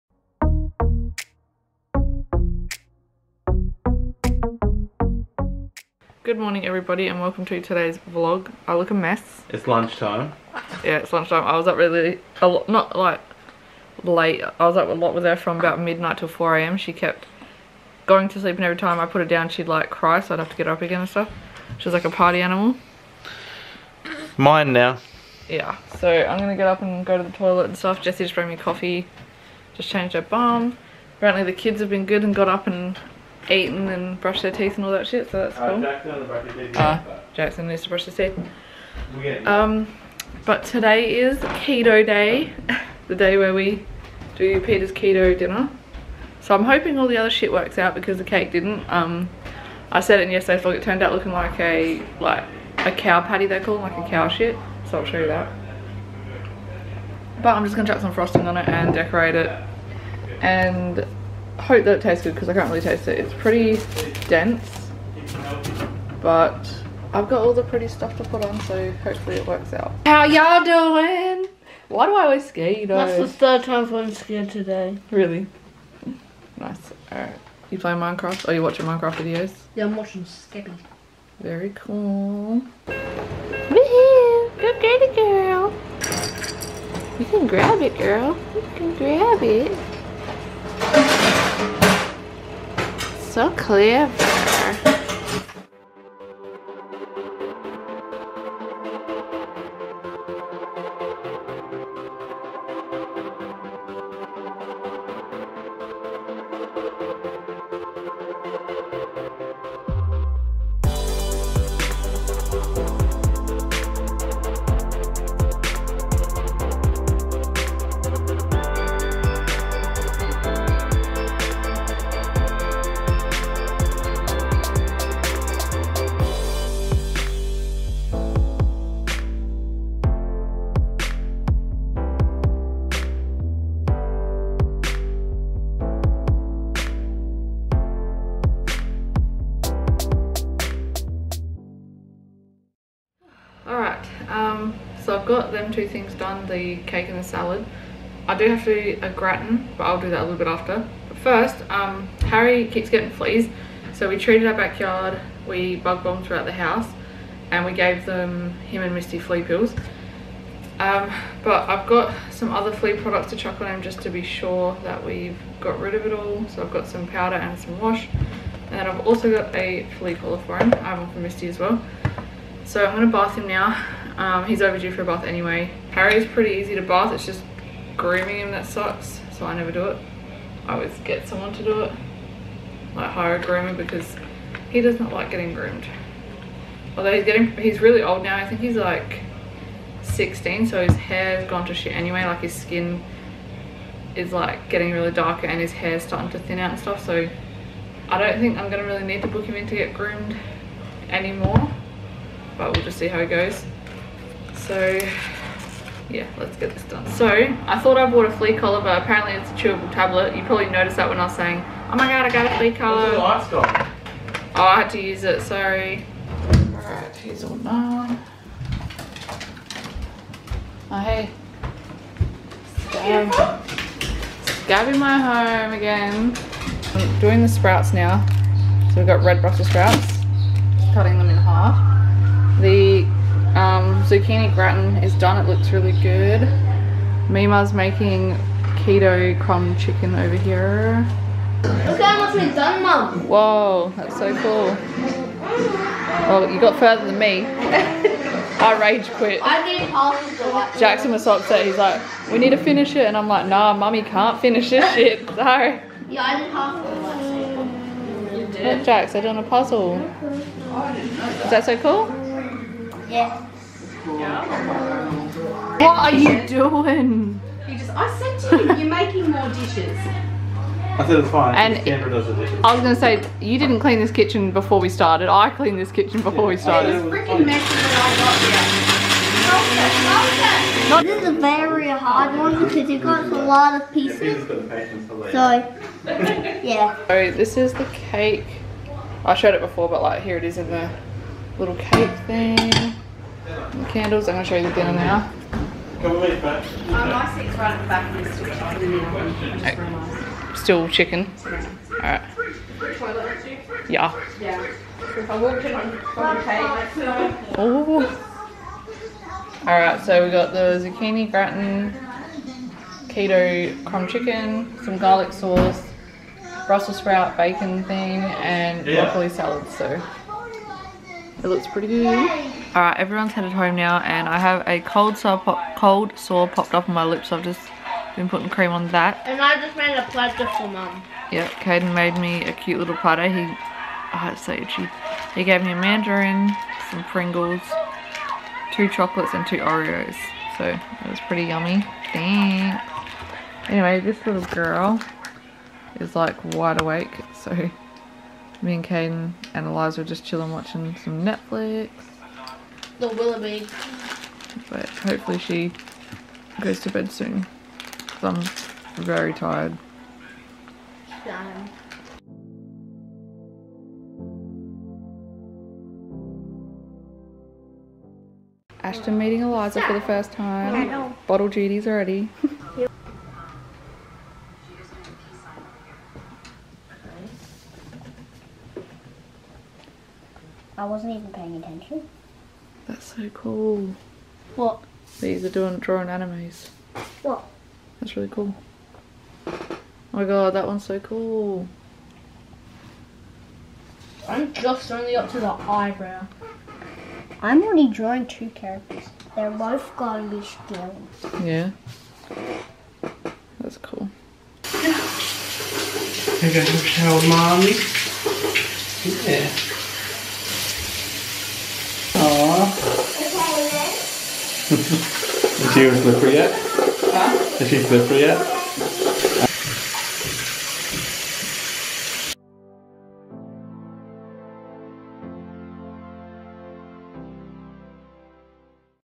KETO DINNER FOR PETA'S BIRTHDAY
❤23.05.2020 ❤

Some early birthday celebrations for Peta
I wanted to cook her a yummy dinner and she is on the Keto diet so i had fun trying out some new recipes

Keandra: kdimoulas
Jesse: jesse997

Whyalla Norrie S.A 5608

❤ NICK'S INSTAGRAM!